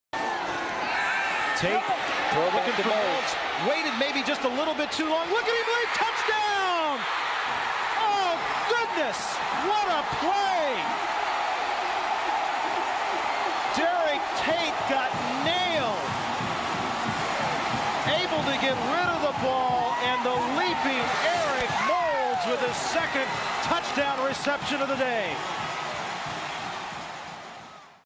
Eric Moulds Takes Flight vs Alabama 1994
Eric Moulds leaps to grab a touchdown pass against Alabama.

All audio and video is property of Mississippi State.